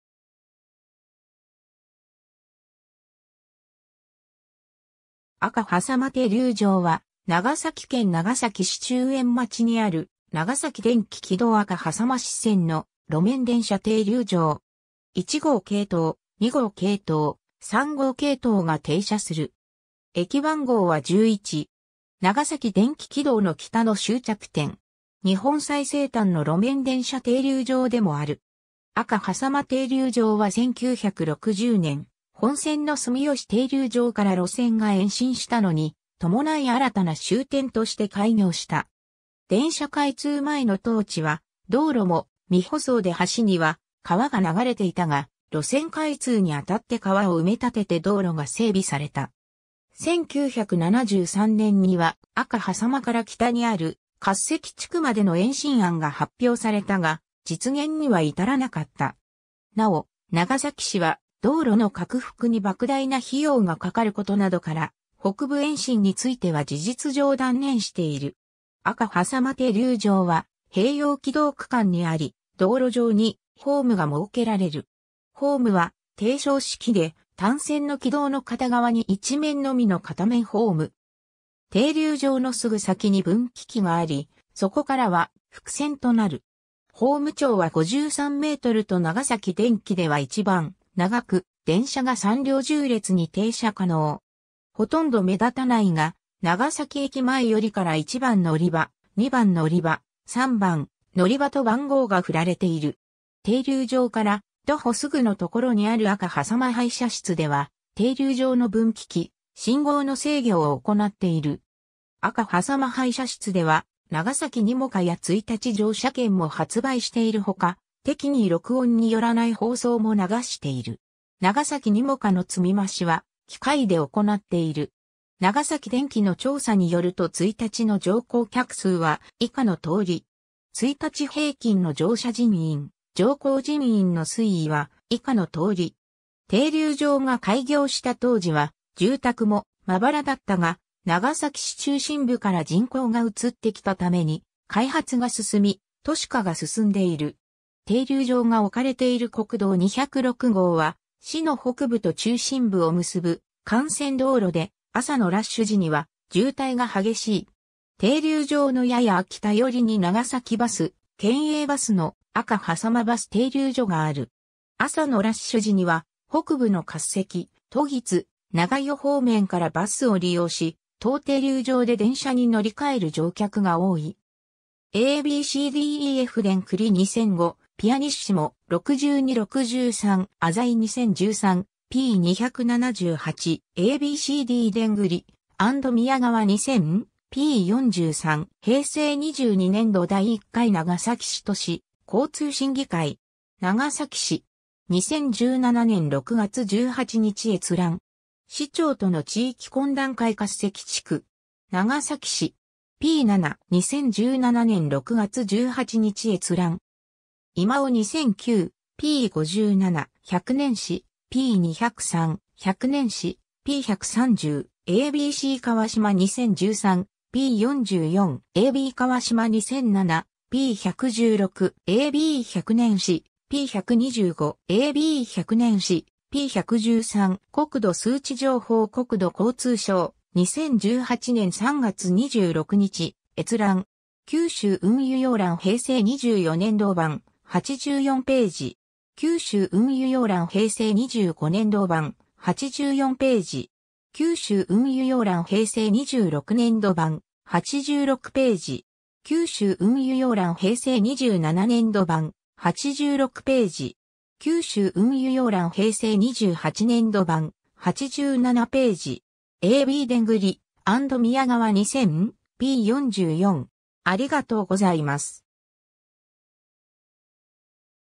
赤迫停留場
赤迫停留場（あかさこていりゅうじょう、赤迫電停）は、長崎県長崎市中園町にある長崎電気軌道赤迫支線の路面電車停留場。
1号系統、2号系統、3号系統が停車する。
 長崎電気軌道の北の終着点。
日本最西端の路面電車停留場でもある。
 赤迫停留場は1960年（昭和35年）、本線の住吉停留場から路線が延伸したのに伴い新たな終点として開業した。
電車開通前の当地は道路も未舗装で端には川が流れていたが、路線開通にあたって川を埋め立てて道路が整備された。
 1973年（昭和48年）には赤迫から北にある滑石地区までの延伸案が発表されたが、実現には至らなかった。
なお、長崎市は道路の拡幅に莫大な費用が掛かることなどから、北部延伸については事実上断念している。
 赤迫停留場は併用軌道区間にあり、道路上にホームが設けられる。
ホームは低床式で、単線の軌道の片側（住吉寄りから見て左側）に1面のみの片面ホーム。
停留場のすぐ先に分岐器があり、そこからは複線となる。
ホーム長は53メートルと長崎電軌では一番長く、電車が3両縦列に停車可能。
ほとんど目立たないが長崎駅前寄りから1番のりば、2番のりば、3番のりばと番号が振られている。
 停留場から徒歩すぐの所にある赤迫配車室では、停留場の分岐器・信号の制御を行っている。
赤迫配車室ではnagasaki nimocaや一日乗車券も発売しているほか、適宜録音によらない放送も流している。
nagasaki nimocaの積み増しは機械で行っている。
 長崎電軌の調査によると1日の乗降客数は以下の通り。
 1日平均の乗車人員・乗降人員の推移は以下の通り。
 停留場が開業した当時は住宅もまばらだったが、長崎市中心部から人口が移ってきたために開発が進み、都市化が進んでいる。
停留場が置かれている国道206号は市の北部と中心部を結ぶ幹線道路で、朝のラッシュ時には渋滞が激しい。
 停留場のやや北寄りに長崎バス・県営バスの「赤迫」バス停留所がある。
朝のラッシュ時には北部の滑石・時津・長与方面からバスを利用し、当停留場で電車に乗り換える乗客が多い。
 ^ a b c d e f 田栗 2005, pp. 62–63. ^ 浅井 2013, p. 278. ^ a b c d 田栗 & 宮川 2000, p. 43. ^ “平成22年度第1回長崎市都市交通審議会”. 長崎市 (2013年3月1日). 2017年6月18日閲覧。
 ^ “市長との地域懇談会 滑石地区 (PDF)”. 長崎市. p. 7. 2017年6月18日閲覧。
 ^ 今尾 2009, p. 57. ^ 100年史, p. 203. ^ 100年史, p. 130. ^ a b c 川島 2013, p. 44. ^ a b 川島 2007, p. 116. ^ a b 100年史, p. 125. ^ a b 100年史, p. 113. ^ 国土数値情報(駅別乗降客数データ) - 国土交通省、2018年3月26日閲覧。
 ^ 『九州運輸要覧 平成24年度版 (PDF) 』、84頁。
 ^ 『九州運輸要覧 平成25年度版 (PDF) 』、84頁。
 ^ 『九州運輸要覧 平成26年度版 (PDF) 』、86頁。
 ^ 『九州運輸要覧 平成27年度版 (PDF) 』、86頁。
 ^ 『九州運輸要覧 平成28年度版 (PDF) 』、87頁。
 ^ a b 田栗 & 宮川 2000, p. 44.